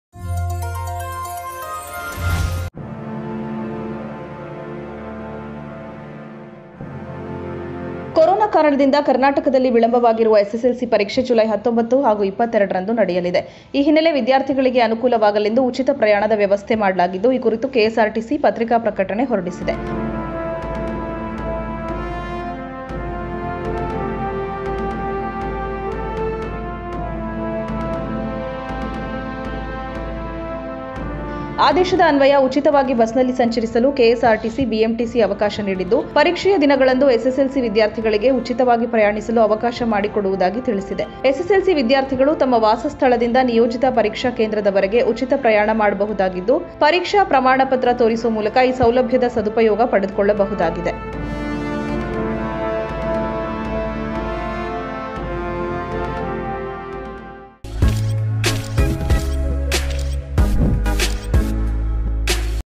SSLC ಪರೀಕ್ಷೆ ಬರೆಯುವ ವಿದ್ಯಾರ್ಥಿಗಳಿಗೆ ಉಚಿತ ಸಾರಿಗೆ ಬಸ್‌ ವ್ಯವಸ್ಥೆ | Free Bus | Oneindia Kannada
ಎಸ್ಎಸ್‌ಎಲ್‌ಸಿ ಪರೀಕ್ಷೆ ಬರೆಯುವ ವಿದ್ಯಾರ್ಥಿಗಳಿಗೆ ಉಚಿತ ಸಾರಿಗೆ ಬಸ್‌ ವ್ಯವಸ್ಥೆ